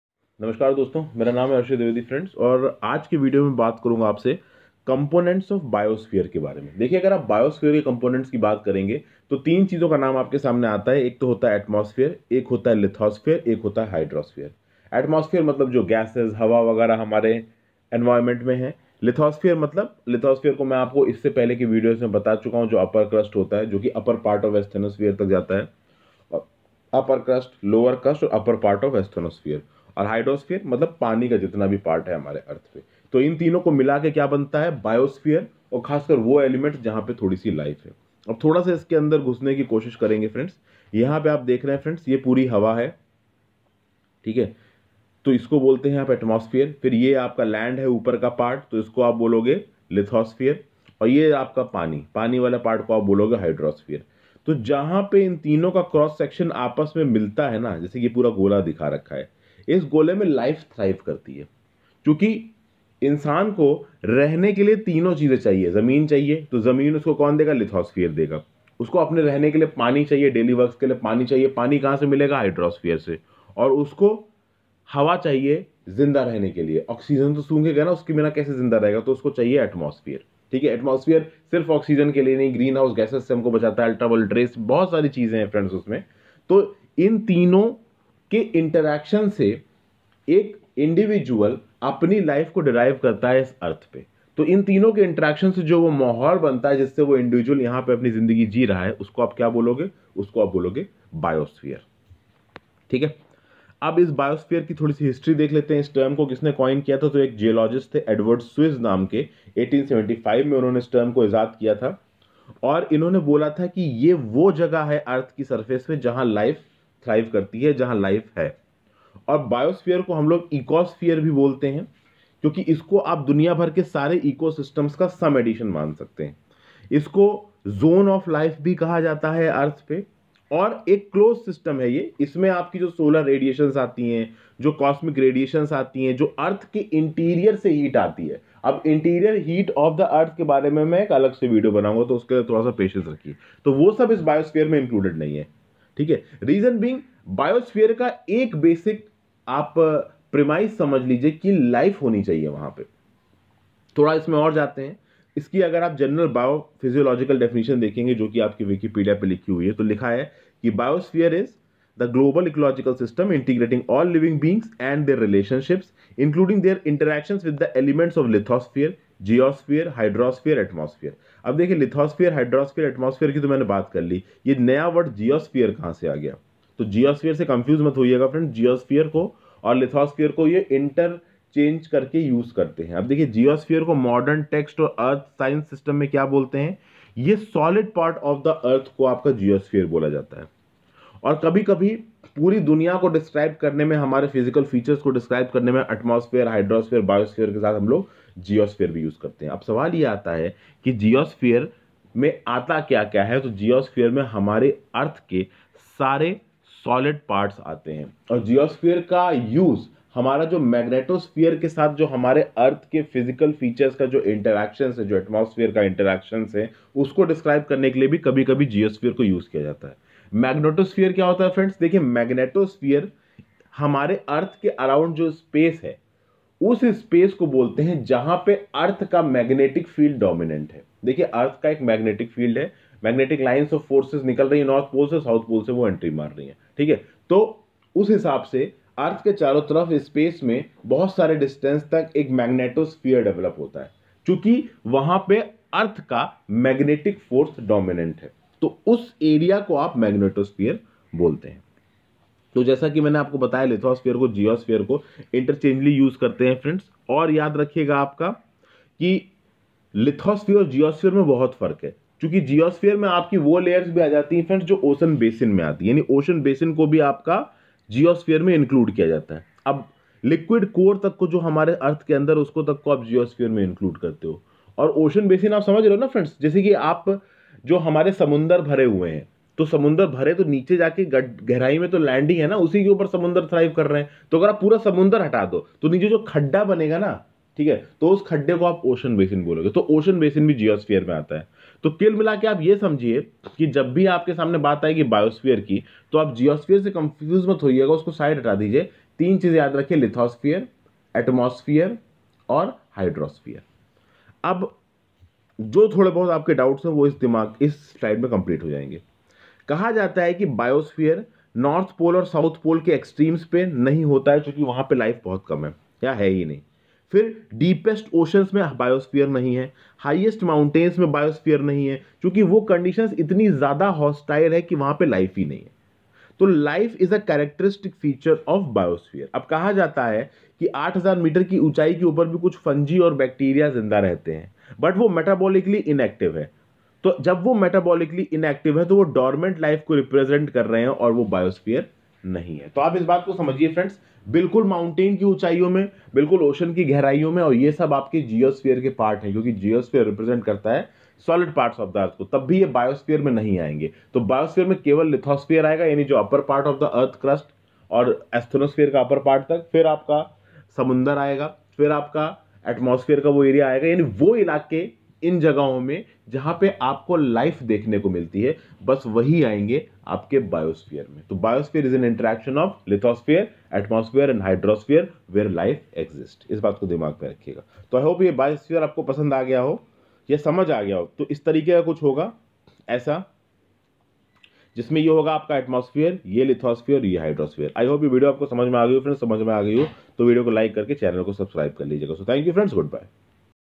Components of Biosphere - Atmosphere, Lithosphere & Hydrosphere (In Hindi)
In this video I have talked about Biosphere also known as the ecosphere which is the combined sum of all ecosystems also termed as the zone of life on Earth as it is formed by the combined interaction of Atmosphere, Hydrosphere and Lithosphere which are air, water and land.